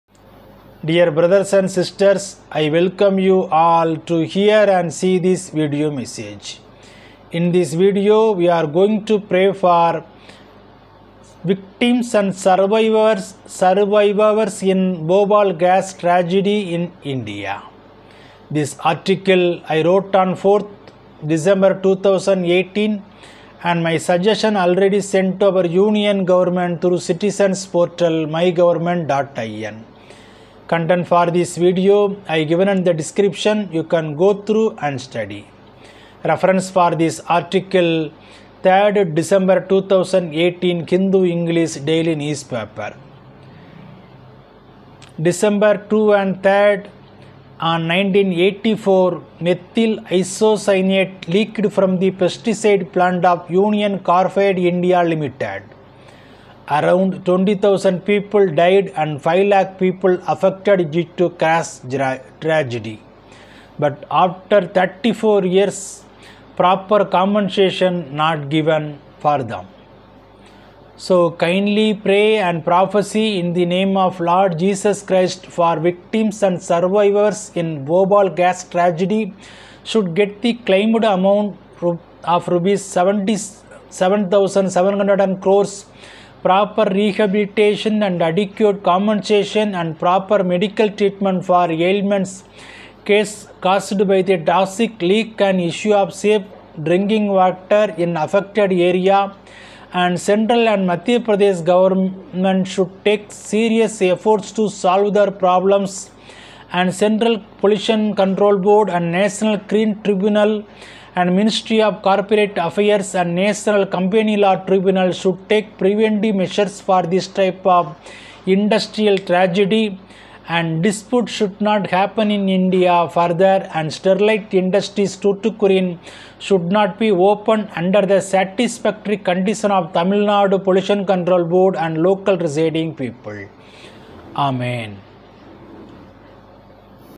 Pray -Victims/Survivors of Bhopal gas tragedy Should get Proper Rehabilitation/ compensation/Justice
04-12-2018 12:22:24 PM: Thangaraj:
 Ref: Hindu-English 03/12/18.
Kindly Pray & Prophesy in the Name of Lord Jesus Christ for Victims and Survivors in Bhopal gas tragedy Should get the claimed amount of Rs 7700 crores, Proper Rehabilitation and adequate compensation and proper medical treatment for ailments caused by the toxic leak and issue of safe drinking water in affected area and Central & Madhya Pradesh government should takes series efforts to solve their problems and Central Pollution Control Board,National Green Tribunal ,Ministry of Corporate Affairs, National Company Law Tribunal should take preventive measures for this type Industrial tragedy /dispute should not happen in India further and Sterlite Industries ( Tutucorin) Should not be open under the satisfactory condition of Tamilnadu Pollution control board and Local Residing People .

( December 2,3 / 1984 Methyl Isocyanate leaked from the pesticide plant of Union Carbide India Limited ( UCIL) .  Around 20,000 people died and 5 Lakh people affected due to gas tragedy. But after 34 years proper compensation not given